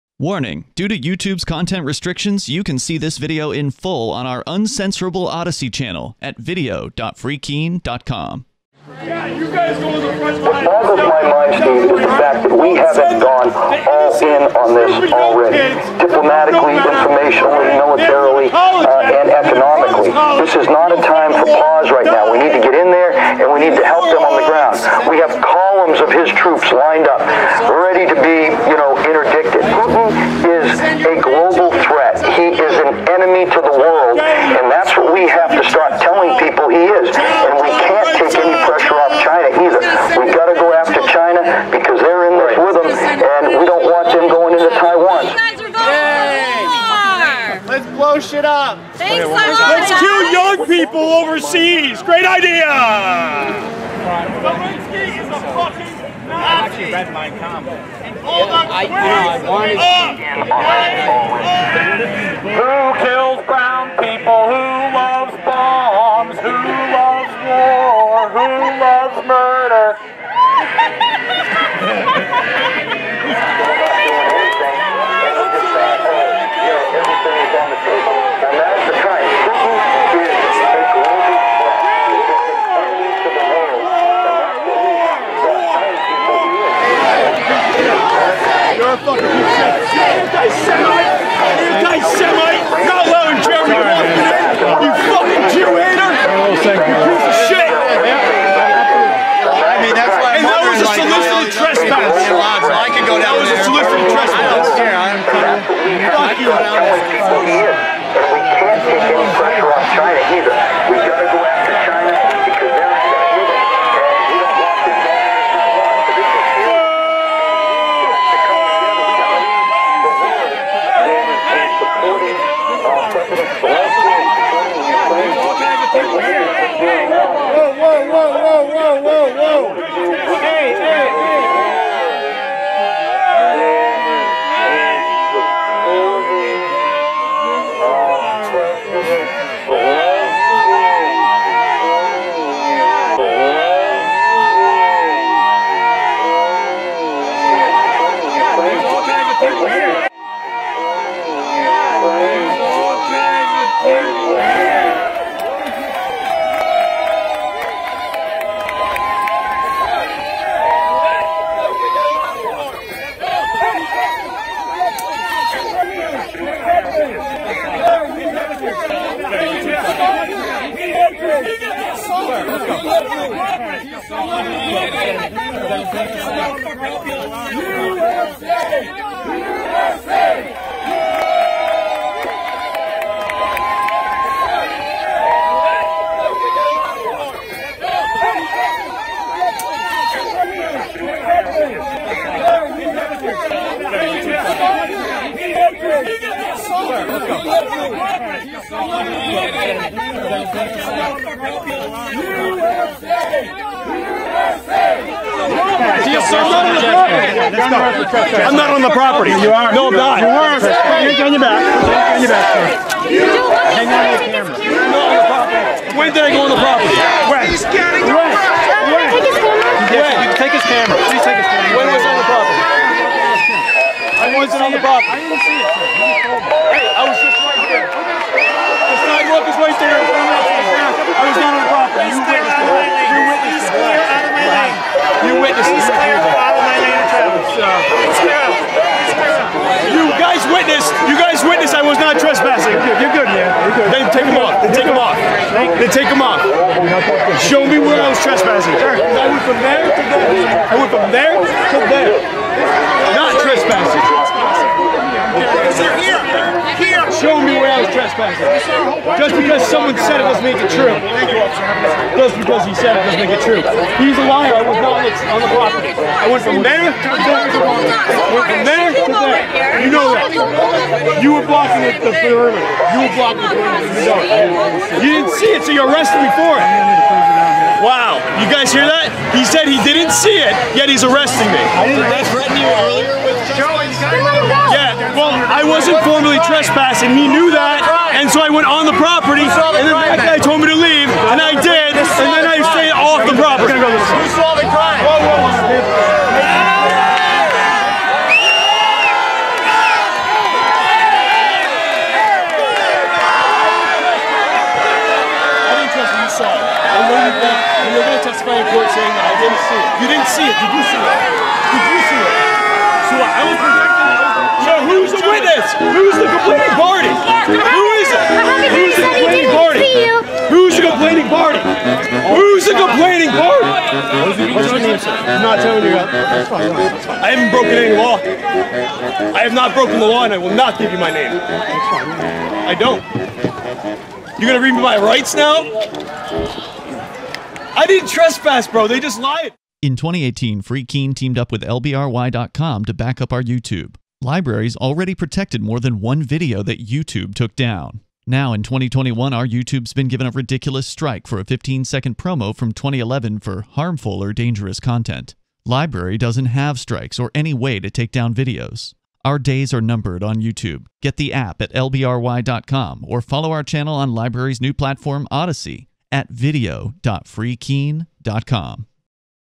Scenes Outside US Senate Debate in NH: Kauffman Protest, Bolduc Lies to have Joa Arrested
Free Keene already published the damning video showing General Don Bolduc, the republican candidate for US senate in NH, lying to police to have Joa from "Breaking the Flaw" wrongfully arrested. The story and video has gone viral yet right wing publications have largely ignored the truth of the video that proves Joa never touched Bolduc. Meanwhile, Joa has filed complaints against Bolduc with Goffstown police, as Bolduc was the one who committed assault against Joa and then made false reports to police.

Now, you can see this longer form video showing various scenes from outside the senate debate at St. Anselm College, including libertarians protesting Jeremy Kauffman being excluded from the debate, but mostly focusing on the entire arrest and subsequent release of Joa, on our Odysee channel here:

 Should be an interesting case when it goes to trial. Joa's arraignment is at Goffstown District Court at 8:15 am on December 1st.